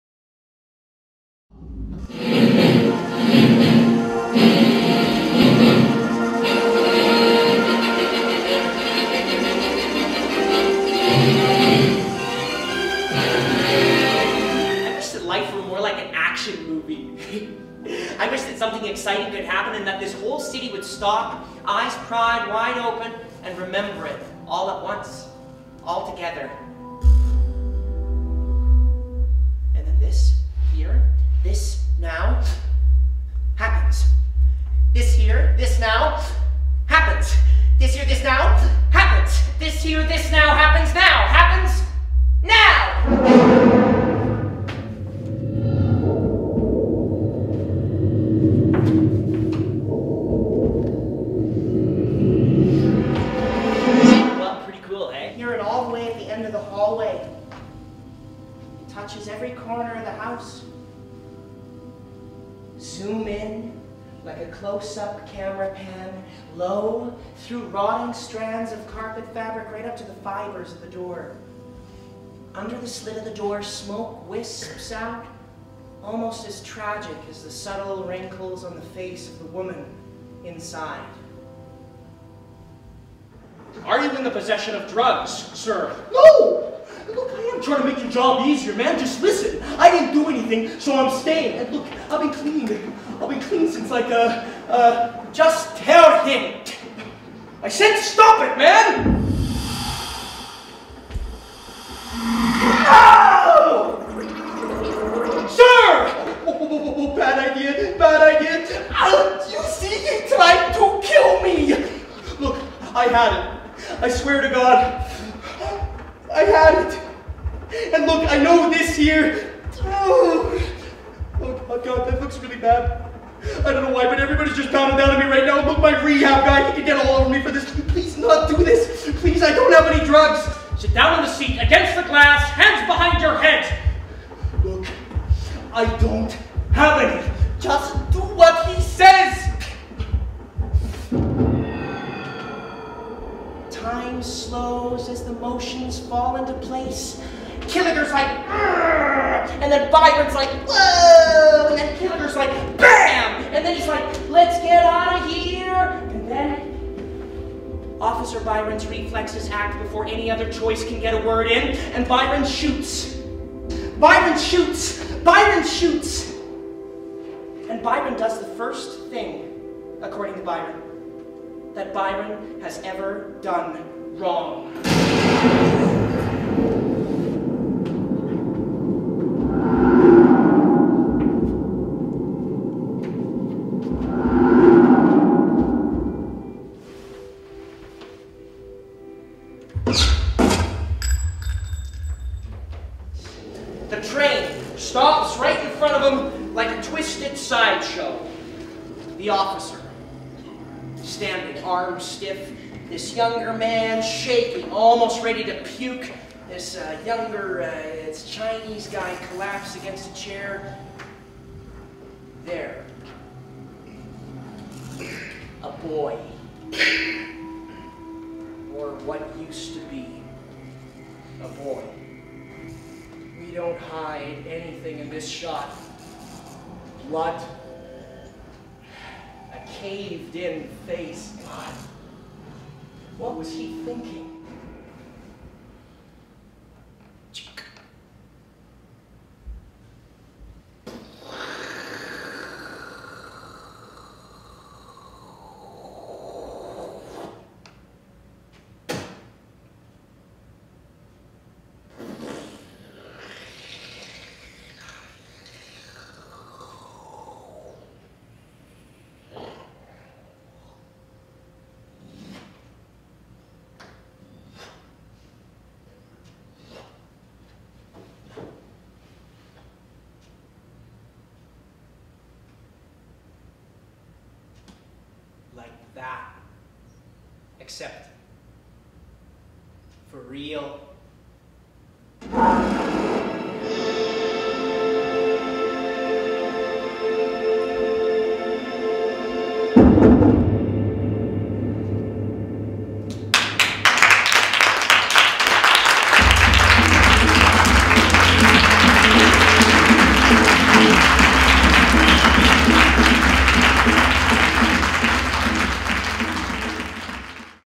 Big Shot, by Jon Lachlan Stewart - 5 minute Sampler
Here's a five minute sample of Surreal SoReal Theatre's production of Big Shot, by Jon Lachlan Stewart. The show has been touring since 2008, but has recently had a big design revamp in the latest 2012 Edmonton Production.